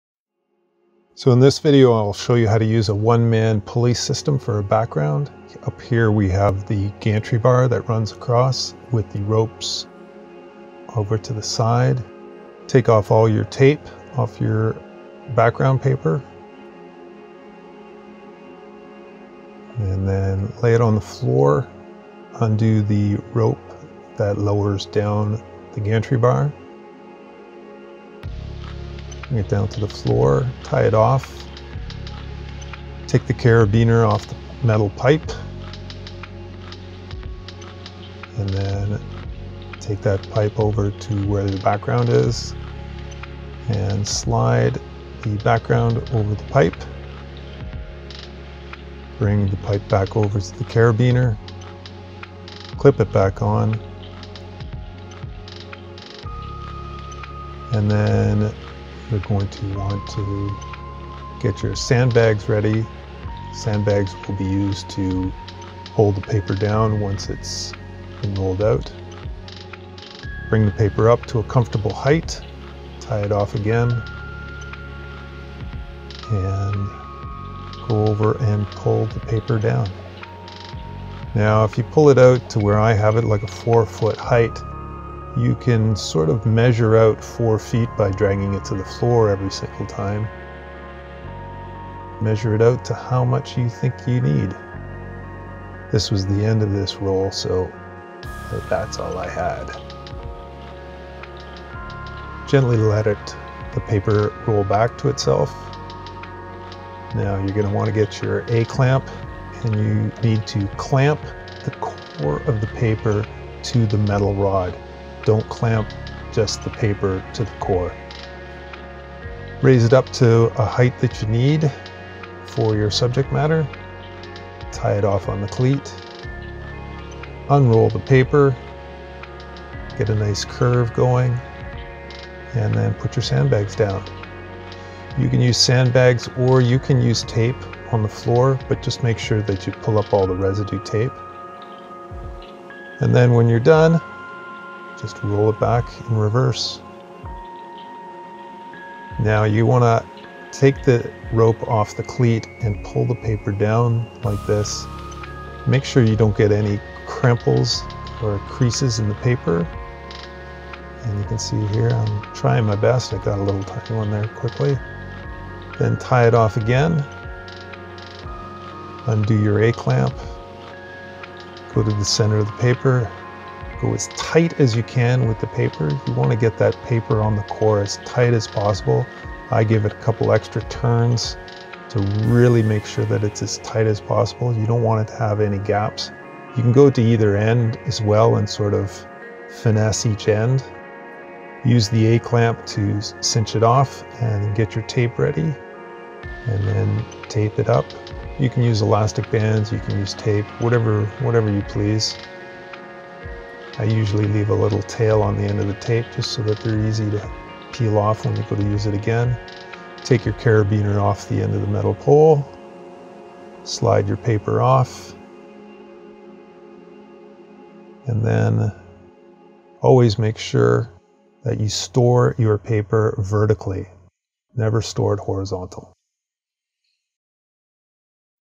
One Person Pulley Background System
How to set up a 9' paper roll on a one person pulley system.